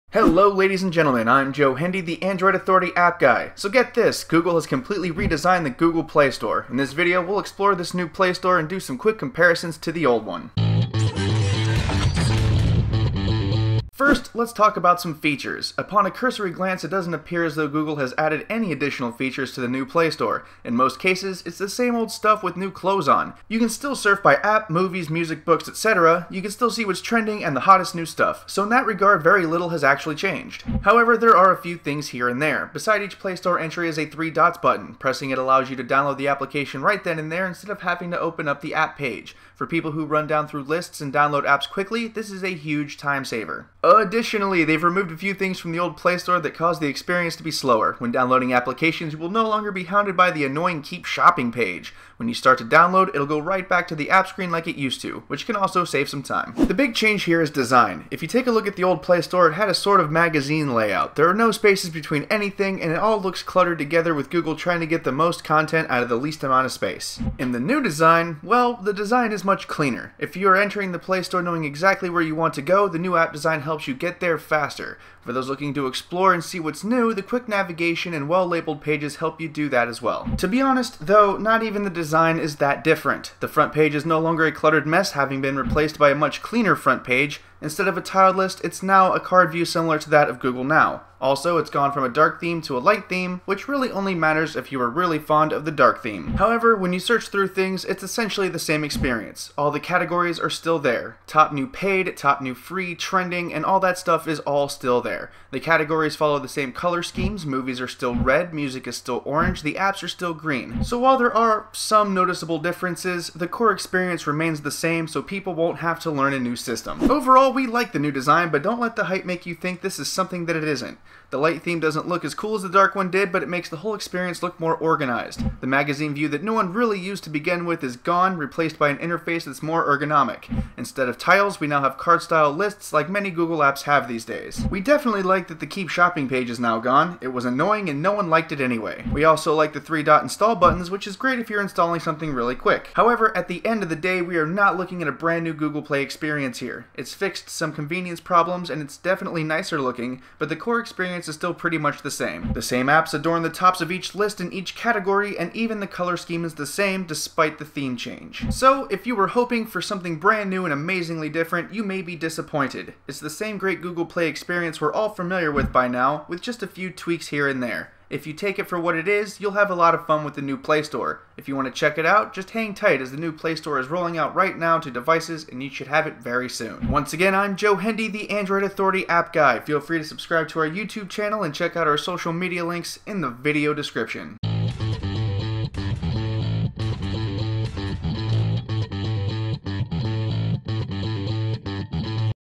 New Google Play Store - Everything You Need to Know!
So Google has released the new Google Play Store and it's rolling out to phones now. So we decided to let everyone know what it's all about.